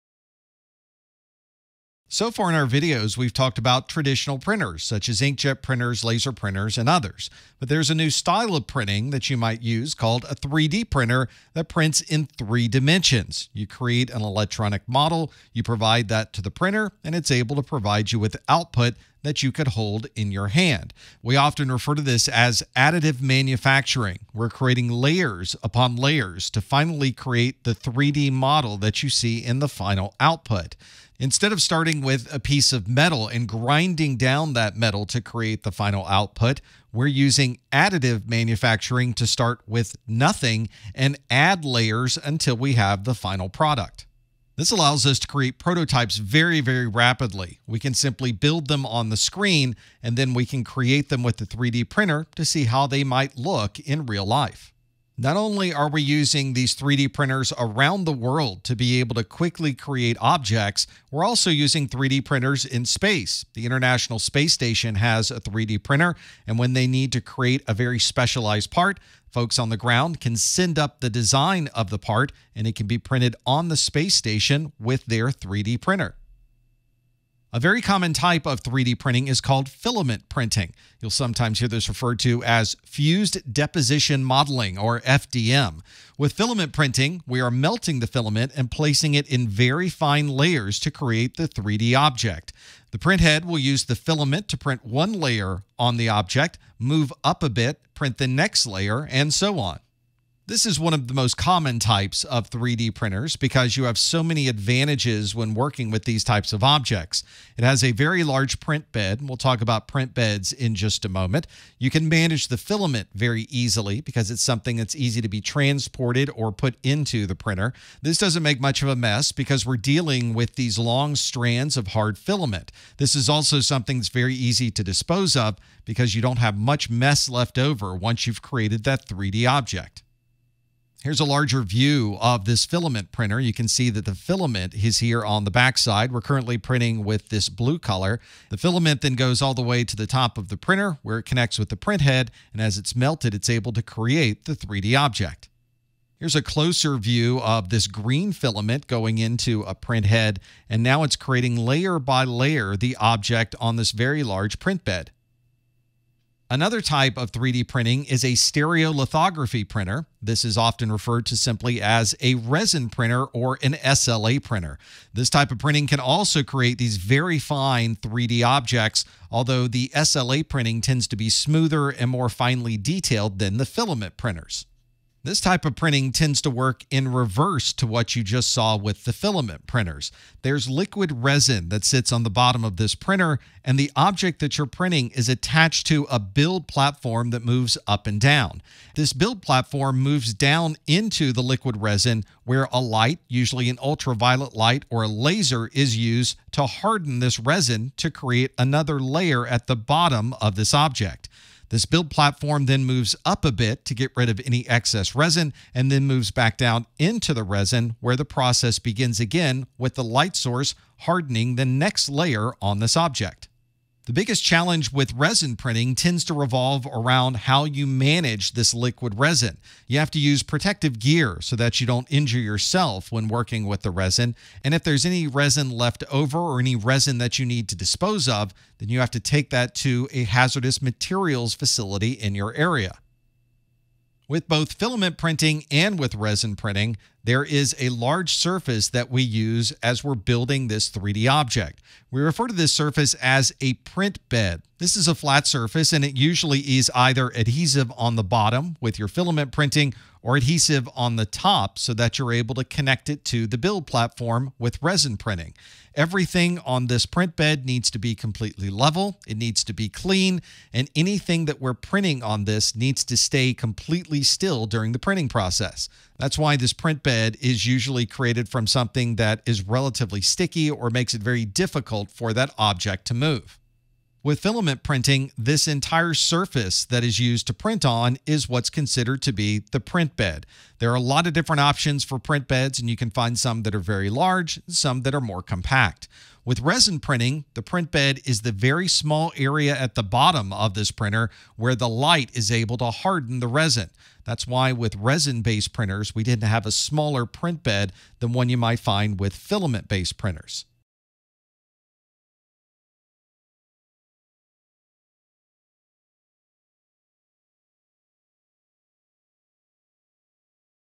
3D Printers - CompTIA A+ 220-1101 - 3.7
3D printers can provide rapid prototyping and object creation. In this video, you’ll learn about filament printing, resin printing, and the print bed of a 3D printer.